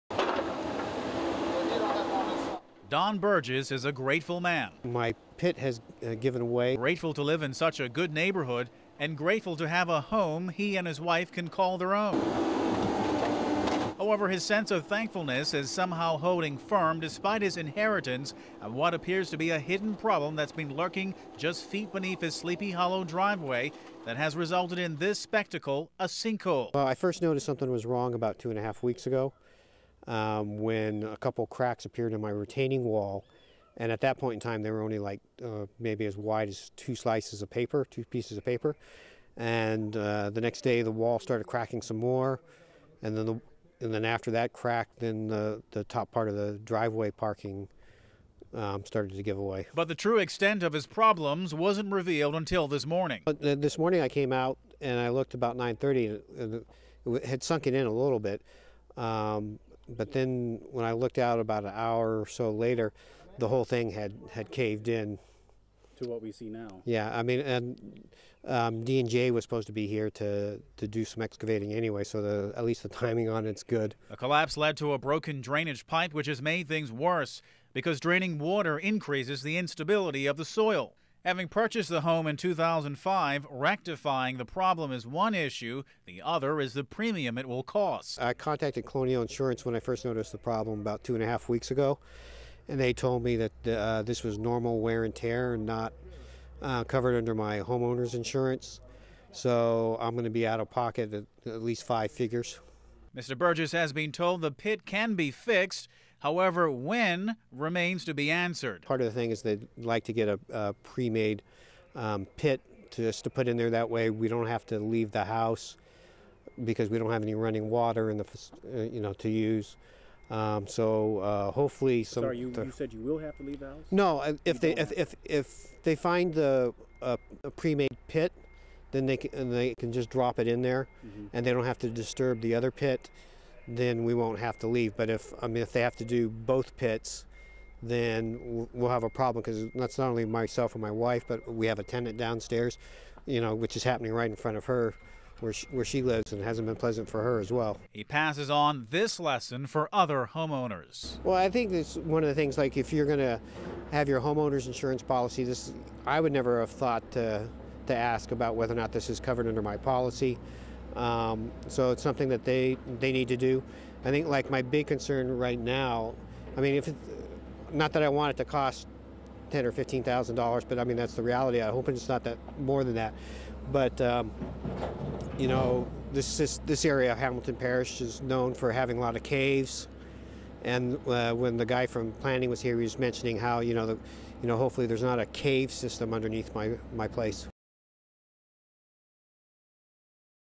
Man finds large sinkhole in his front yard
HOME-OWNERS beware... how well do you know your property? It's a question not only for you, but for potential home-buyers as well.

It concerns a Hamilton Parish man who faces a hefty bill following the discovery of a massive sink-hole on his driveway.

News 9's Tari Trott reports.